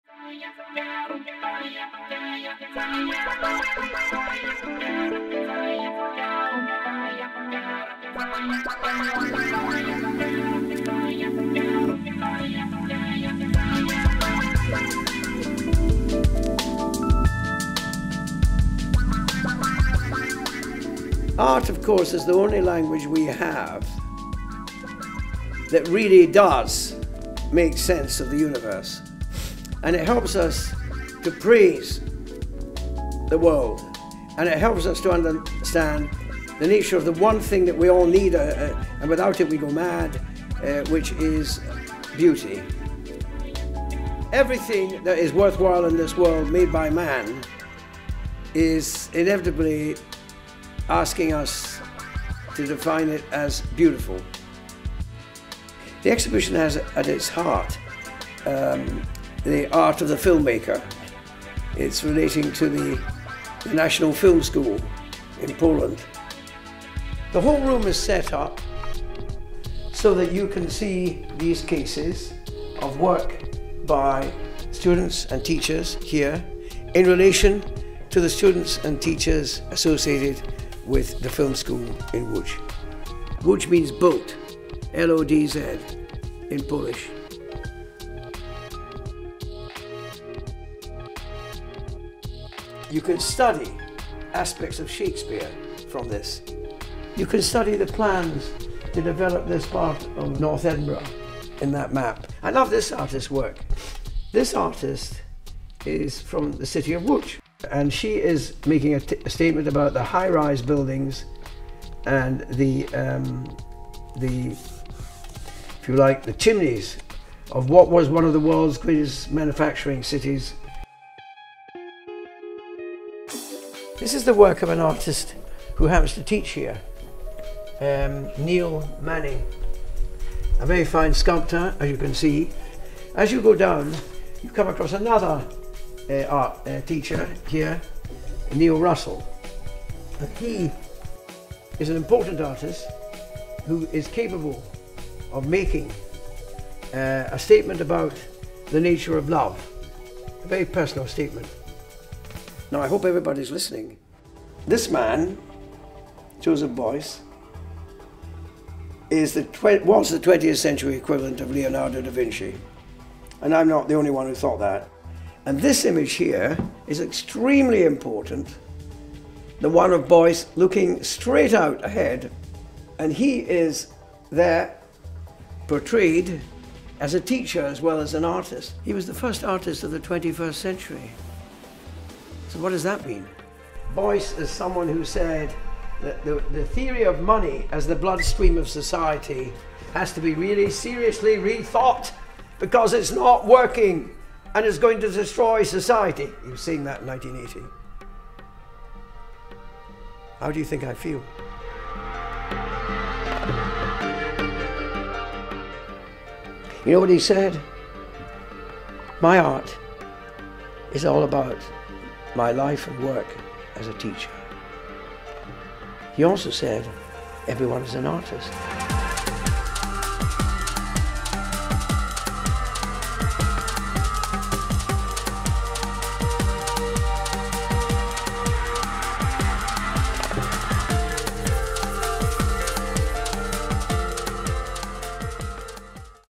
Richard Demarco Art Exhibition
This Is my final factual program for my HND: Creative Industries Television at Edinburgh's Telford College.
It is a piece on an exhibition put on by Richard Demarco showing the work of students and teachers from telford college as well as students and teachers from the National Film School in Poland.
Thanks